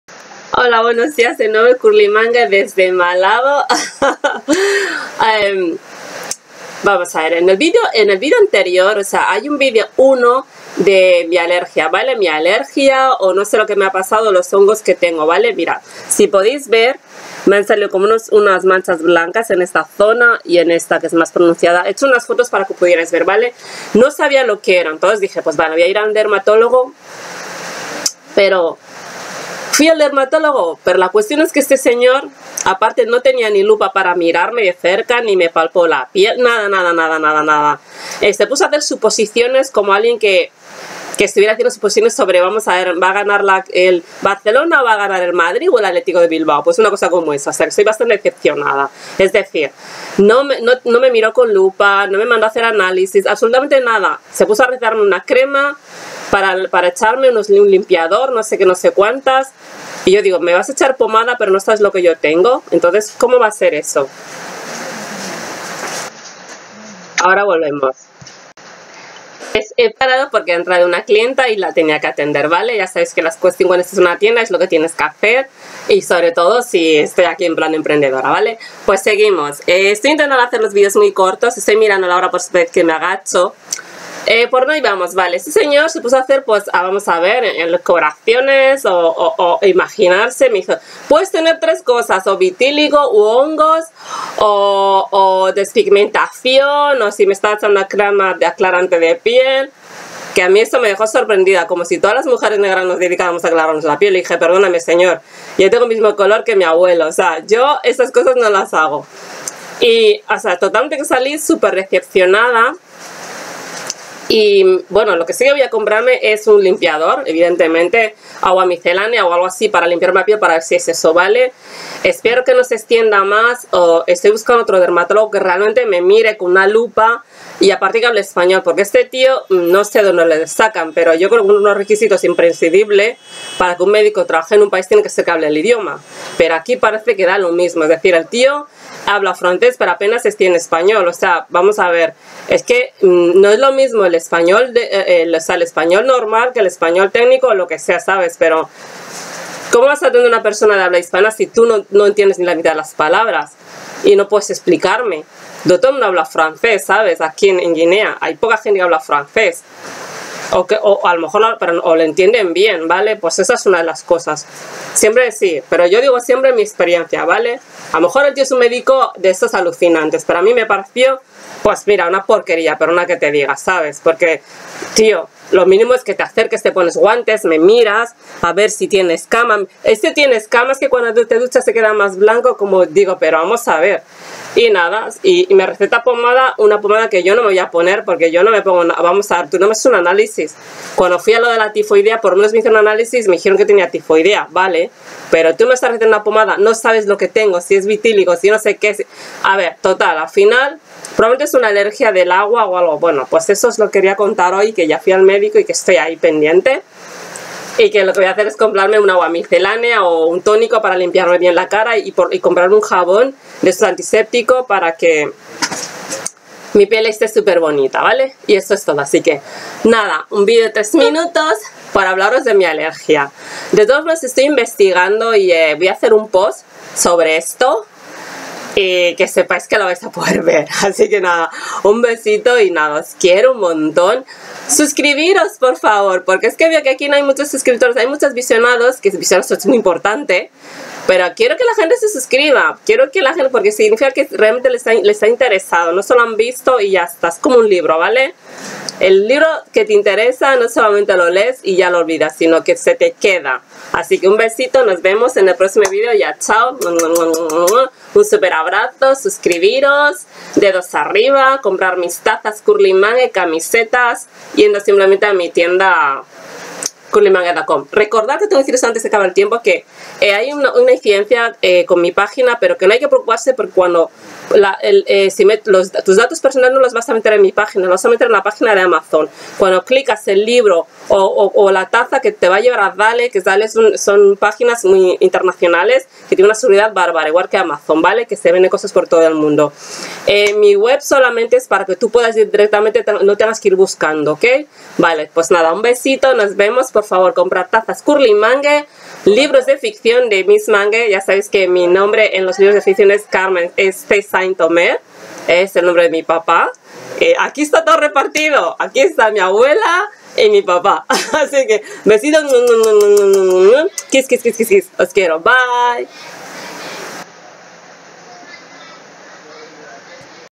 Un dermatólogo etíope que juega a la quiniela con mi diagnóstico: hongos en la piel
En este video explico  la experiencia que tuve con un mal dermatólogo  al que acudí. Para mi sorpresa se puso a jugar a la loteria con mi alergia a  suponer lo que tenia en lugar de mirarme. Ni Siquiera  se digo a mirar mi con con la lupa ni a tocarme. Un dermatólogo etíope  negro  que trata a los negros  como si fuera un nazi que ha caído por casualidad en a África.